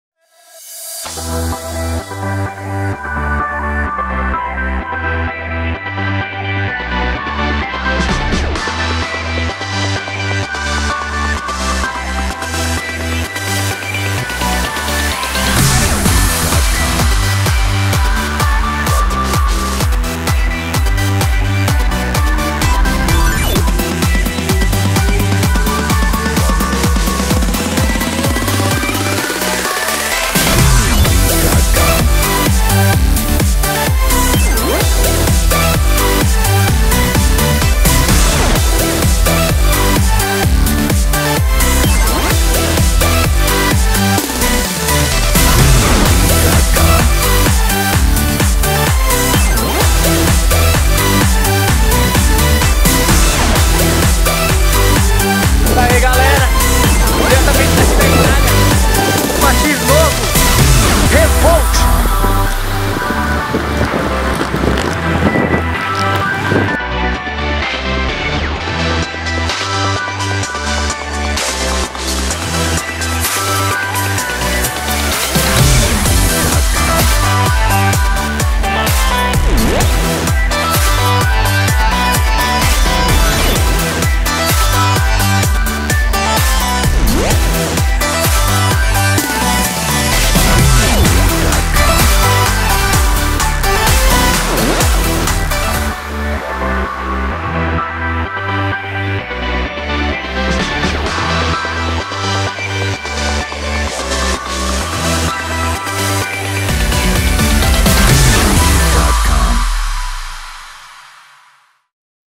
Patins Inline Traxart Revolt
O Patins Revolt foi desenvolvido para práticas de Urban e Freestyle. Como essas modalidades de alto impacto, este patins leva a combinação das melhores peças, proporcionando uma excelente patinação estilo URBAN. Destaque pelo seu design, performance e conforto.
Acompanhe o Revolt em ação neste rolê com nosso atleta Caio Radical.